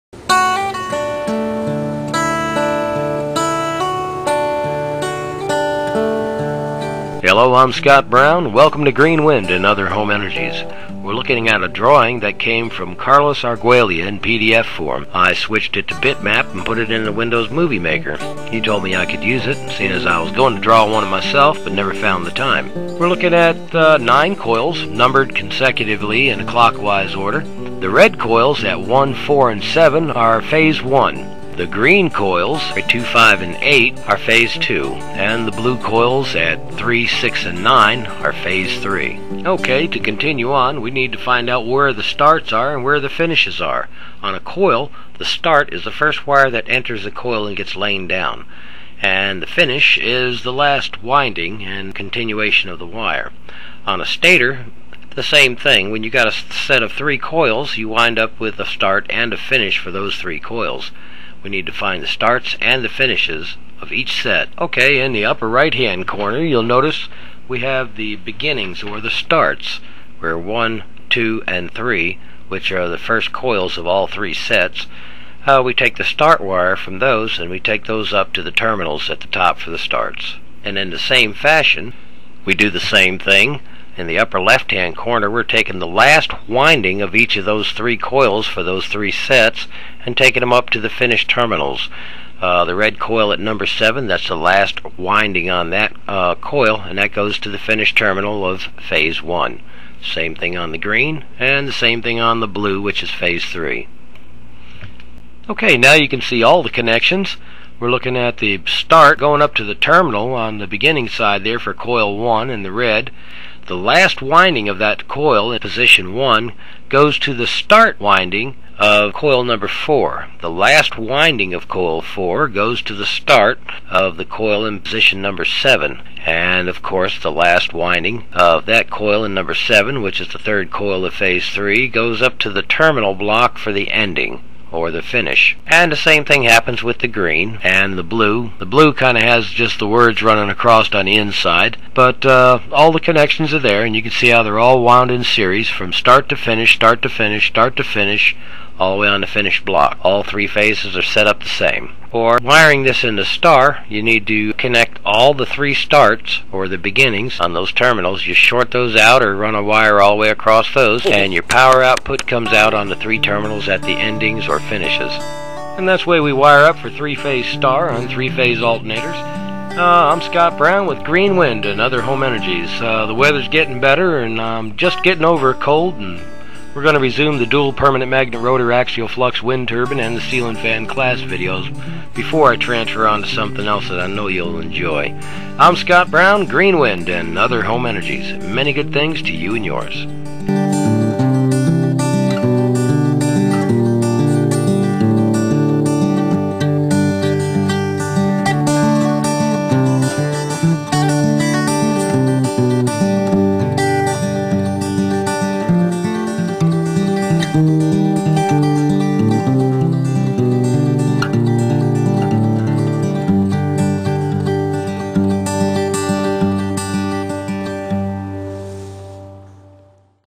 STAR WIRING FOR 3 PHASE STATORS
SO MANY PEOPLE KEEP ASKING ME HOW TO WIRE A STATER IN STAR CONFIGURATION. UNTIL NOW I HAVE BEEN SO BUSY TRYING TO KEEP UP WITH WORK AND ALL, THAT I HARDLY HAD TIME FOR ANYTHING. AND IT IS VERY HARD TO EXPLAIN BY JUST WORDS IN AN EMAIL. A PICTURE SAYS A THOUSAND WORDS, SO PICTURES I NEED.
   THEN ONE OF MY FREQUENT FRIENDS OF YOU TUBE, SENT ME A PDF SET OF DRAWINGS, AND ASKED IF HE HAD IT DOWN RIGHT.
     WWWEEEEHHHAAAWWW! NOT ONLY DID HE HAVE IT RIGHT!, BUT HE HAD BEAUTIFULLY PREPARED DRAWINGS AND ALL. CARLOS WAS VERY HAPPY TO LET ME SHARE HIS WORK WITH YA'LL. BUT FIRST I HAD TO TRANSFER IT FROM PDF, TO BMP. SOMEHOW THE WINDOWS PROGRAM MADE IT BLURRY. I FOUND AN ONLINE CONVERTER. AND WENT TO WORK.
    SO THANKS TO CARLOS ARGUELLO WAY DOWN IN SOUTH AMERICA, I DIDNT HAVE TO FIND THE TIME TO DRAW THESE OUT, AND I WAS ABLE TO TRANSFER A BETTER EXPLANATION FOR STAR WIRING TO YOUR VIEWING PLEASURE AND YOUR EASY STUDY EDUCATION STYLE.
   GOD BLESS YA'LL AND CARLOS TOO.
SCOTTY B.
(MUDDy)